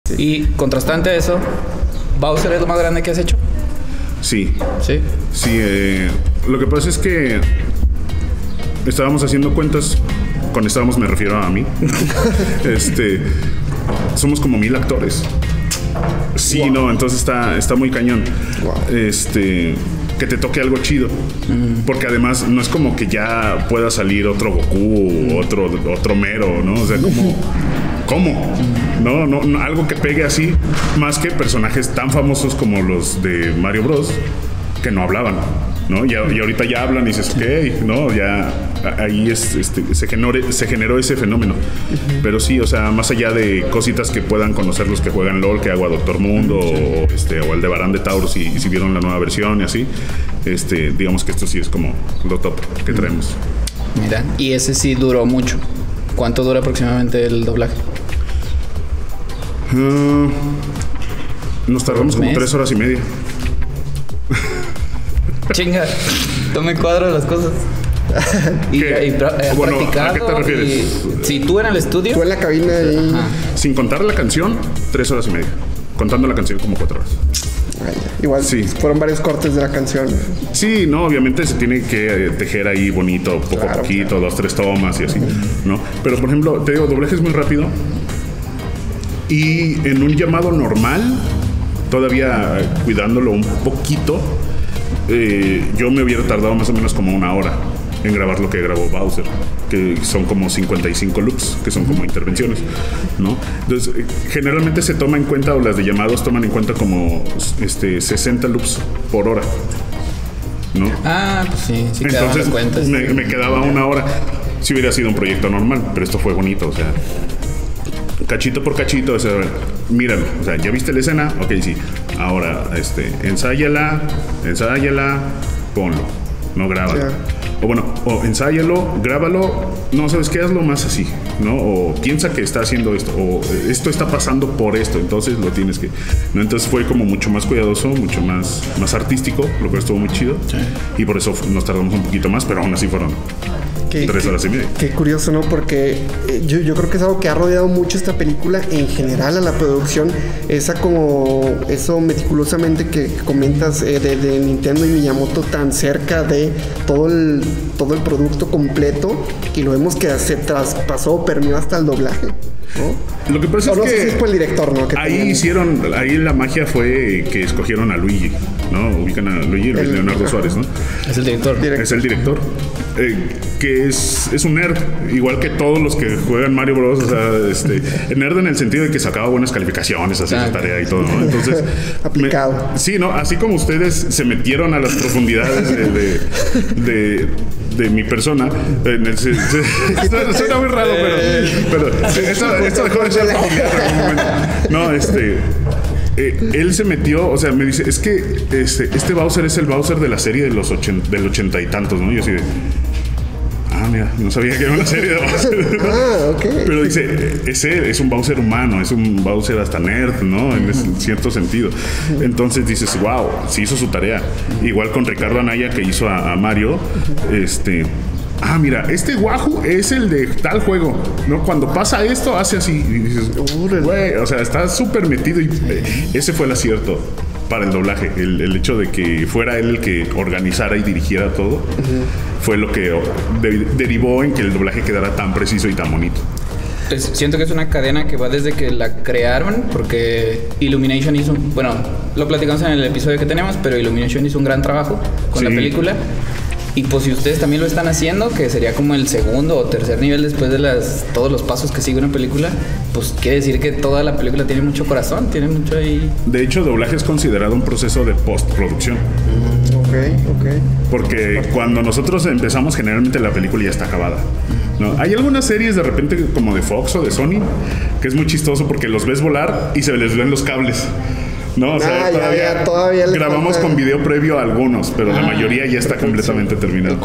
Cuánto tiempo tomó grabar la voz de Bowser? - Héctor Estrada (Voz de Bowser)
Héctor se sienta a platicar con nosotros, y nos cuenta cuánto tiempo tardó en grabar la voz del villano más famoso del universo de Mario Bros.
No imaginábamos para nada su respuesta, y tú?